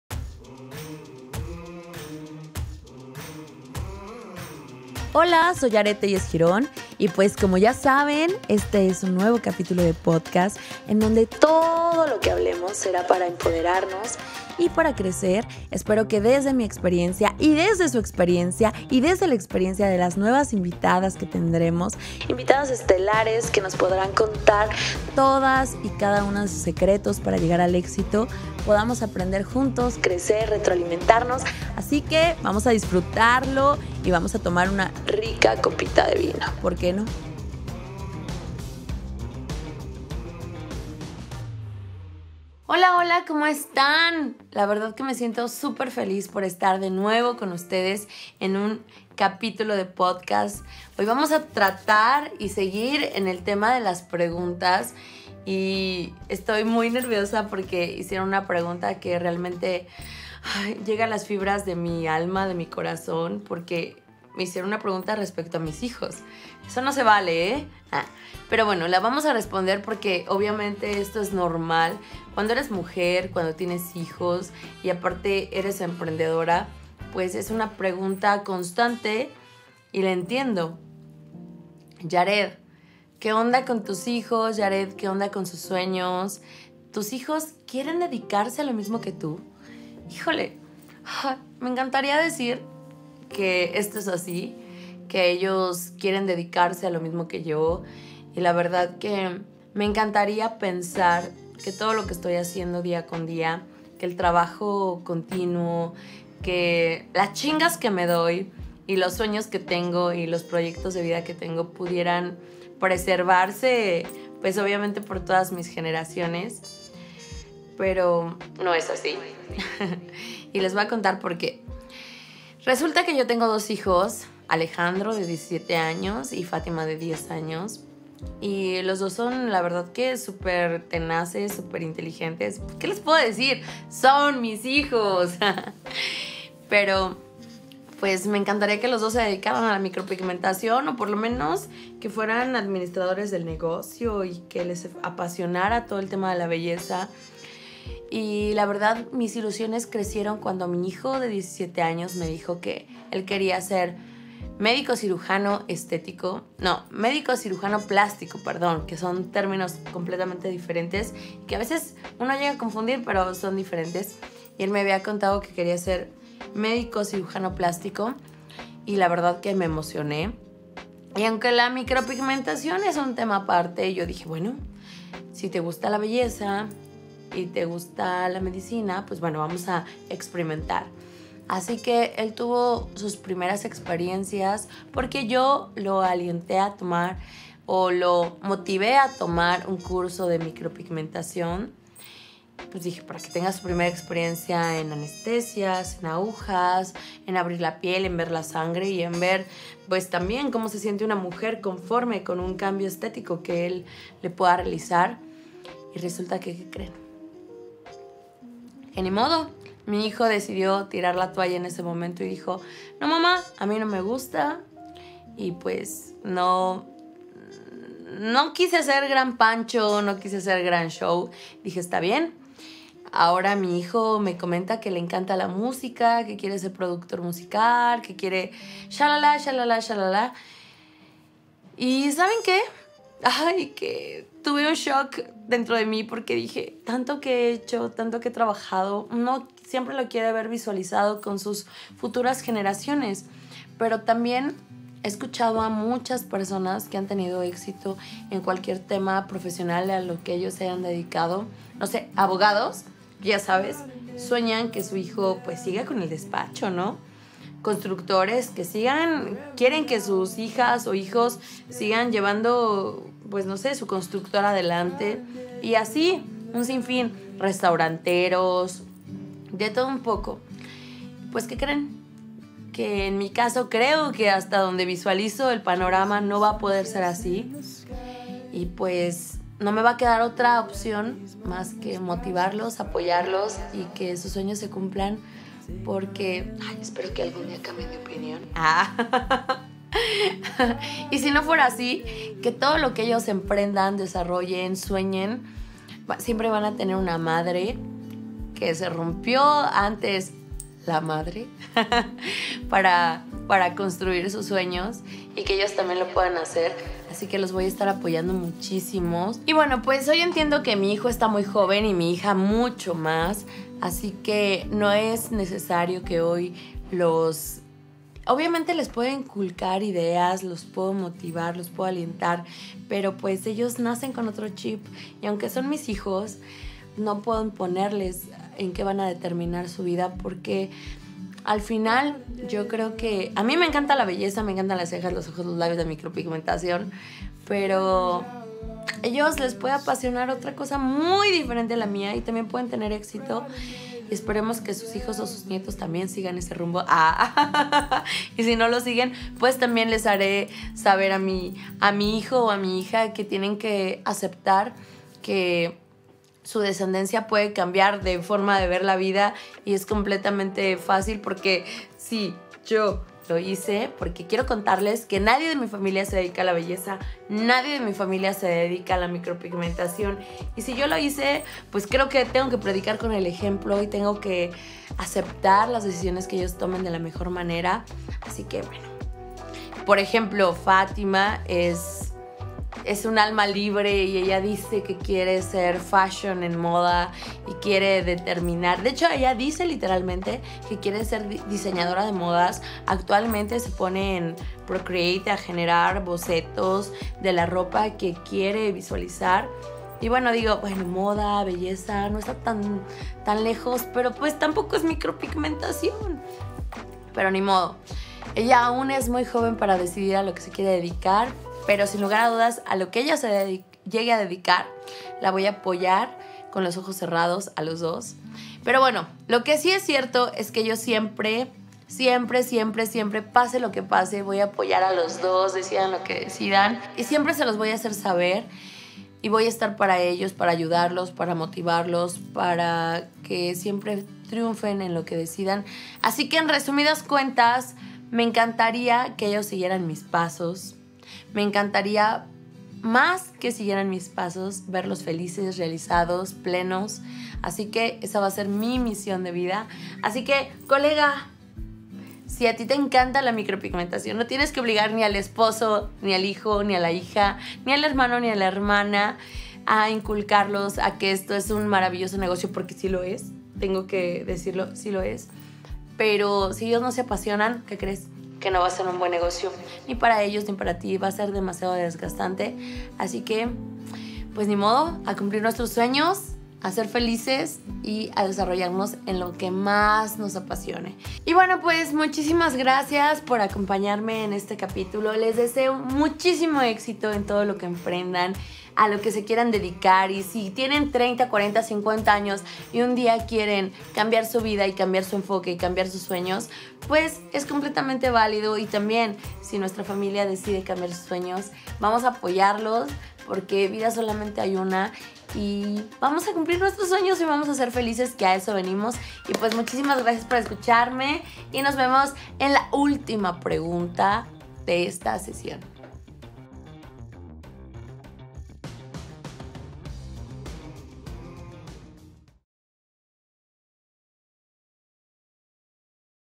Mis hijos mi misión de vida || Podcast
Contestando ya las últimas preguntas del año, y en esta ocasión tocaron un tema profundo y que la verdad es un tema muy común entre platicas, pues uno como madre siempre quiere que sus hijos sigan sus pasos, pero debemos entender que ellos son seres únicos con sueños únicos y que nuestra mayor misión es apoyarlos para que ellos se puedan realizar en lo que ellos deseen.